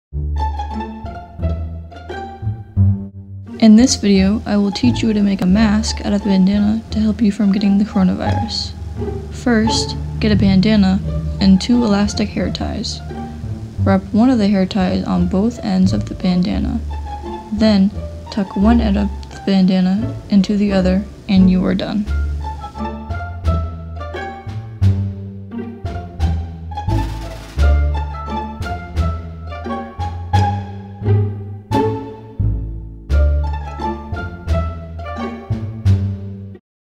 How To Video home made mask making
Video production class 2020.  Students were asked to make a short video explaining how to make a mask at home to wear if they are going out during the covid 19 stay at home order.